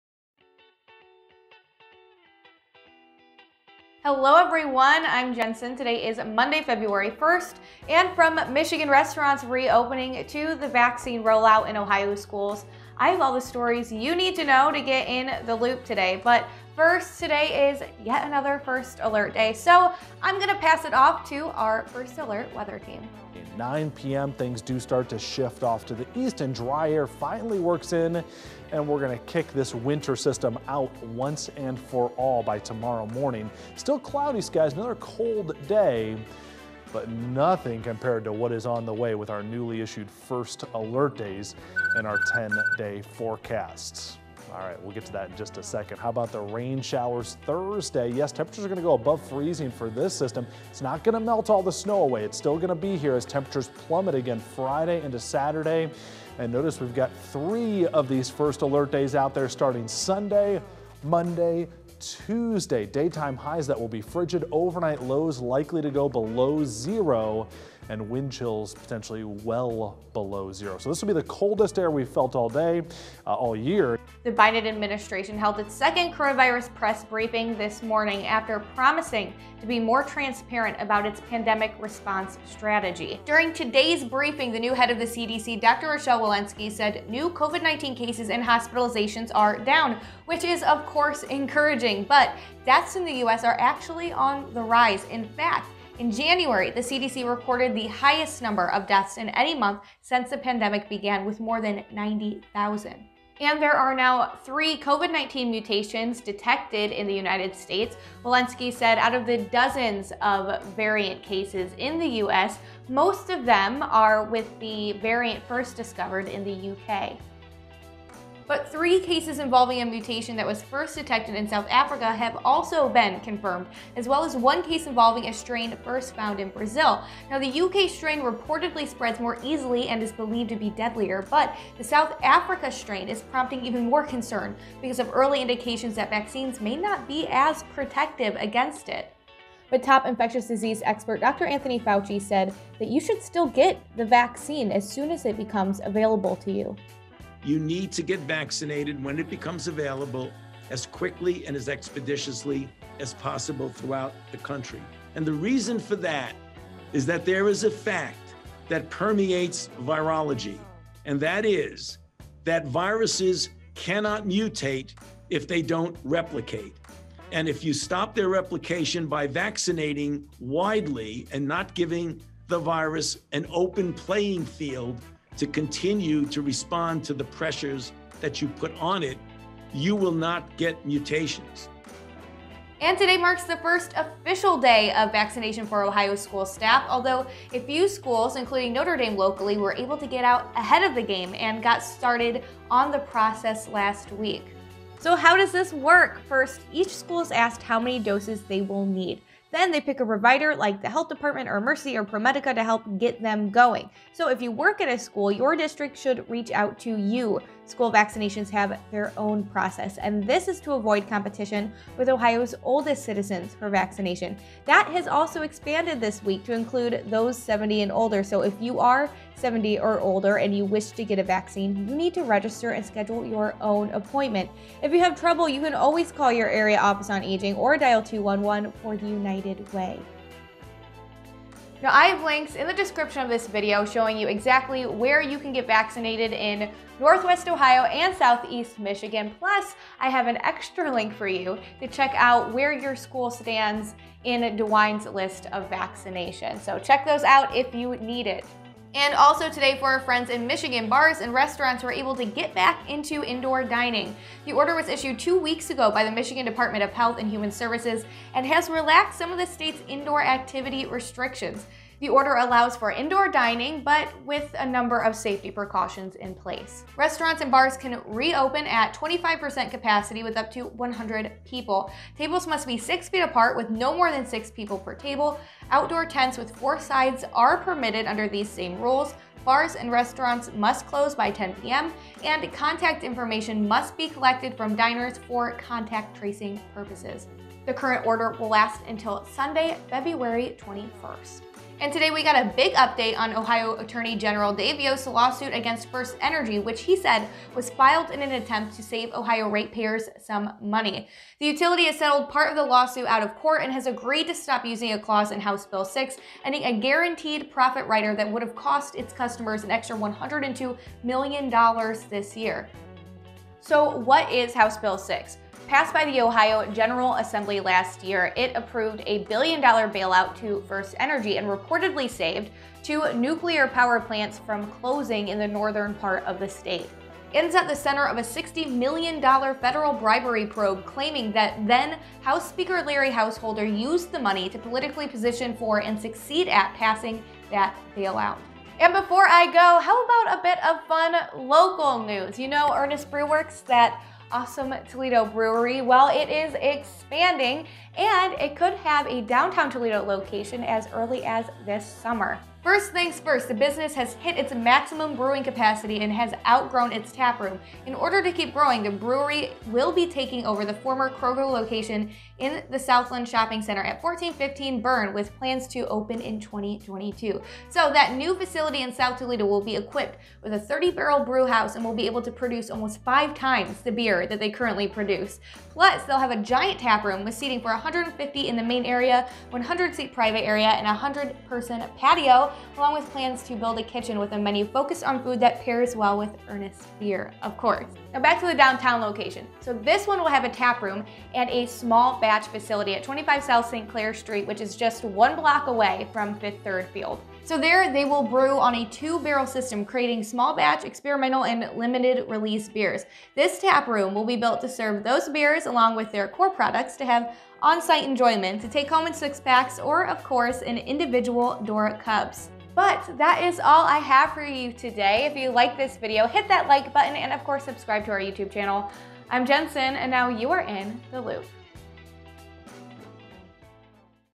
Michigan restaurants reopen, Ohio rolls out COVID-19 vaccines in schools & more | In the Loop: 2/01
Bars and restaurants open for in-person dining in Michigan today, Ohio school workers start to see the COVID-19 vaccine, Toledo brewery Earnest Brew Works plans to expand and more of your top headlines from Feb. 1.